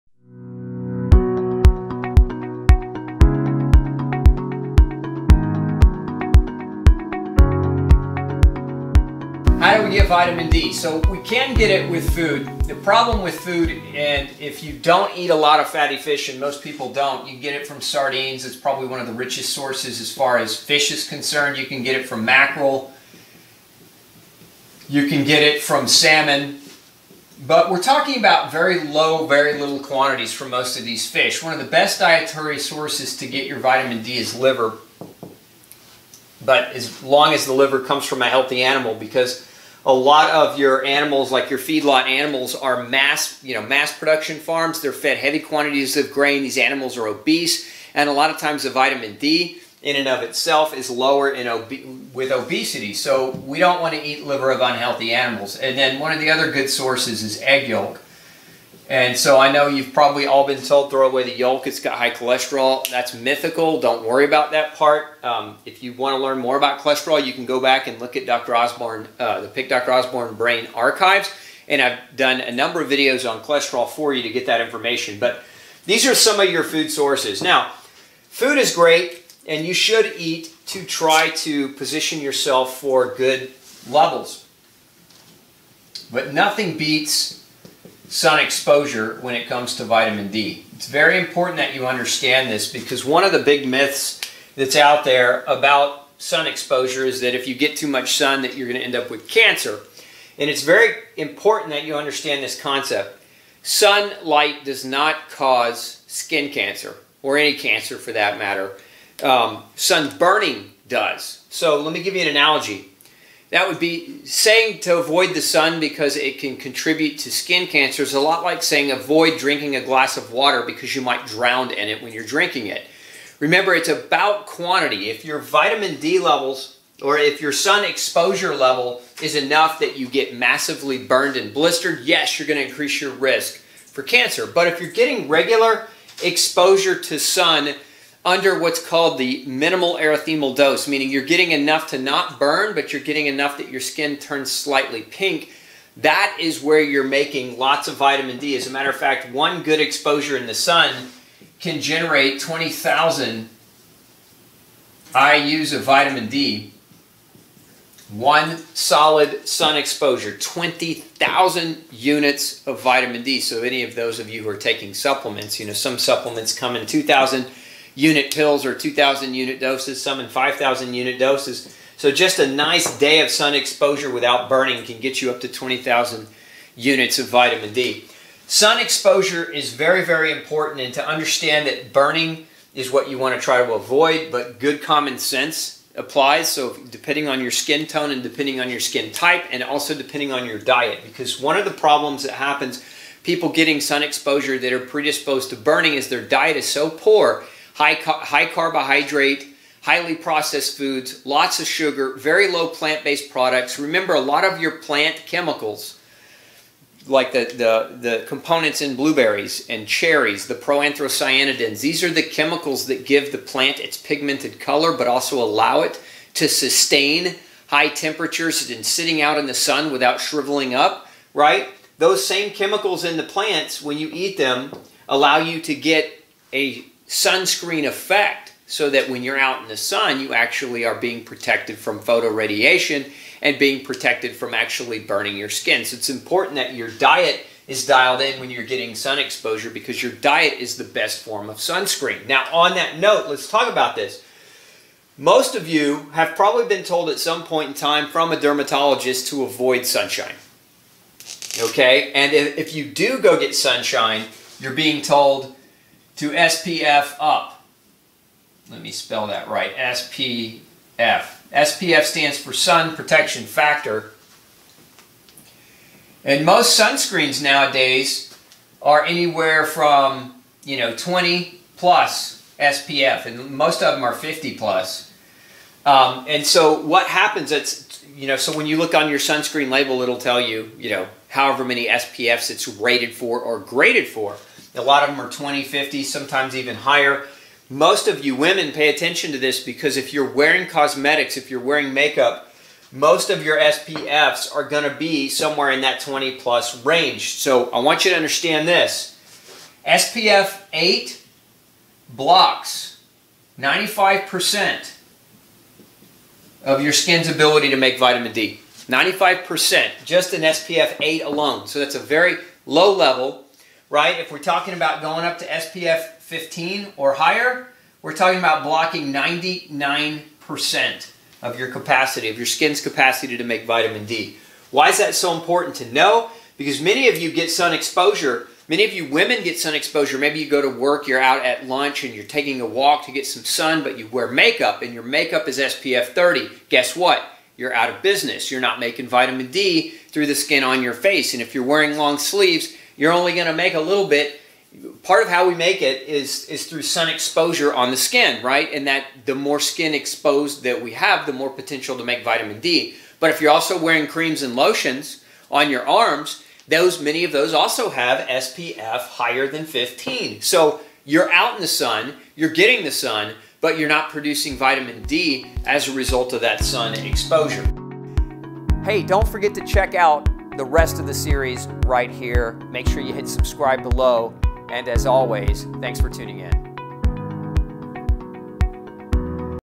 Vitamin D - What is the best way to get it?
What is the best way to get vitamin D?
Sunshine, and lots of it.  Most foods contain low levels of vitamin D, but your skin can make it when exposed to the sun.  Now if you are looking for a food source of vitamin D, check out fatty fish and liver.  Both are good sources of vitamin D.